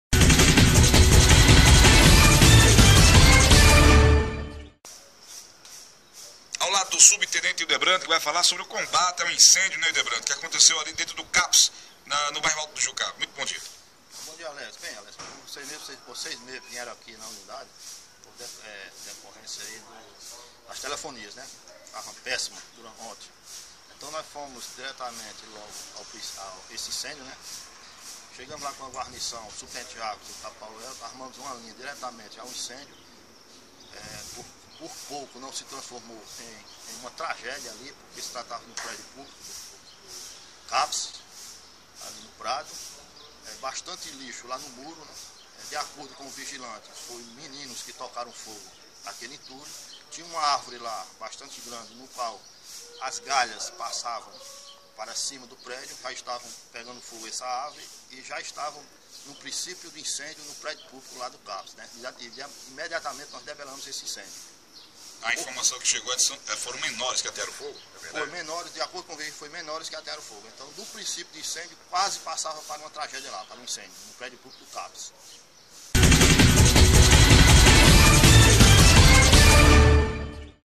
TV Iguatu.Net: Adolescentes ateiam fogo dentro do CAPS de Iguatu-CE
O vandalismo de um grupo de jovens quase termina em tragédia no final da noite de ontem (16) em Iguatu. Segundo o vigia do Centro de Atenção Psicossocial -- CAPS, alguns adolescentes que passavam pela parte de trás do prédio, atearam fogo em um amontoado de papel que estava no interior do CAPS, situado a na Rua 27 de Novembro, bairro Alto do Jucá. As chamas logo se espalharam, atingindo uma árvore e a fiação elétrica.